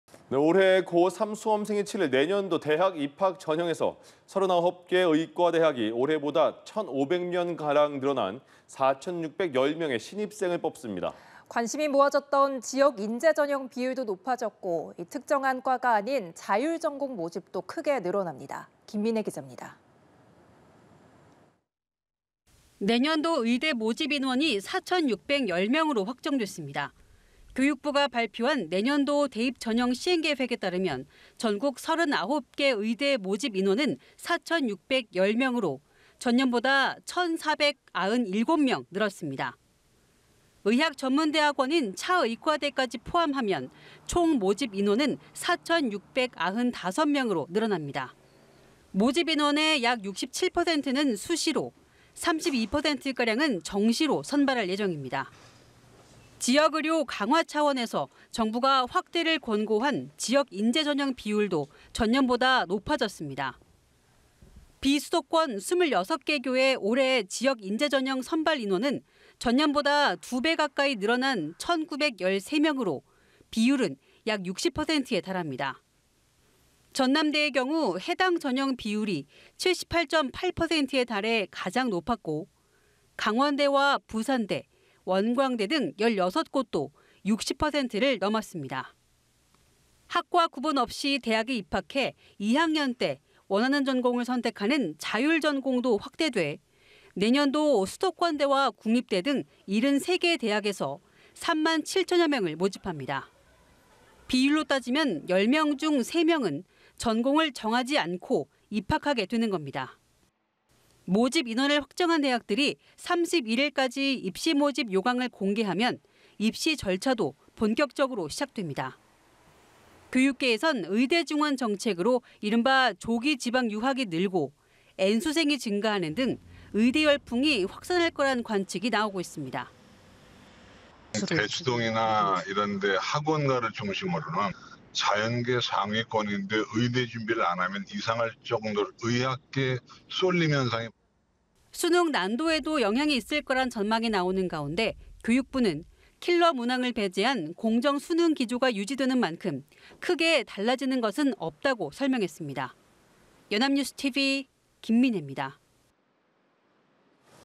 내년 의대정원 1,497명 증가…지역인재전형 비율도 높아져 / 연합뉴스TV (YonhapnewsTV)
내년 의대정원 1,497명 증가…지역인재전형 비율도 높아져
[뉴스리뷰]

[앵커]

올해 고3 수험생이 치를 내년도 대학 입학전형에서 39개 의과대학이 올해보다 1,500명가량 늘어난 4,610명의 신입생을 뽑습니다.

관심이 모아졌던 지역인재전형 비율도 높아졌고, 특정한 과가 아닌 '자율전공' 모집도 크게 늘어납니다.

김민혜 기자입니다.

[기자]

내년도 의대 모집인원이 4,610명으로 확정됐습니다.

교육부가 발표한 내년도 대입전형 시행계획에 따르면 전국 39개 의대 모집인원은 4,610명으로 전년보다 1,497명 늘었습니다.

의학전문대학원인 차의과대까지 포함하면 총 모집인원은 4,695명으로 늘어납니다.

모집인원의 약 67%는 수시로, 32%가량은 정시로 선발할 예정입니다.

지역의료 강화차원에서 정부가 확대를 권고한 지역인재전형 비율도 전년보다 높아졌습니다.

비수도권 26개교의 올해 지역인재전형 선발인원은 전년보다 2배 가까이 늘어난 1,913명으로, 비율은 약 60%에 달합니다.

전남대의 경우 해당 전형 비율이 78.8%에 달해 가장 높았고, 강원대와 부산대, 원광대 등 16곳도 60%를 넘었습니다.

학과 구분 없이 대학에 입학해 2학년 때 원하는 전공을 선택하는 '자율전공'도 확대돼 내년도 수도권대와 국립대 등 73개 대학에서 3만 7천여명을 모집합니다.

비율로 따지면 10명 중 3명은 전공을 정하지 않고 입학하게 되는 것입니다.

모집인원을 확정한 대학들이 31일까지 입시모집 요강을 공개하면, 입시절차도 본격적으로 시작됩니다.

교육계에선 의대 증원 정책으로 이른바 '조기 지방유학'이 늘고 N수생이 증가하는 등 의대 열풍이 확산할 거란 관측이 나오고 있습니다.

[이만기 / 유웨이교육평가연구소장] "대치동이나 이런 데 학원가를 중심으로는 자연계 상위권인데 의대 준비를 안 하면 이상할 정도로 의학계 쏠림 현상이…."

수능 난도에도 영향이 있을 거란 전망이 나오는 가운데 교육부는 킬러문항을 배제한 '공정수능' 기조가 유지되는 만큼 크게 달라지는 것은 없다고 설명했습니다.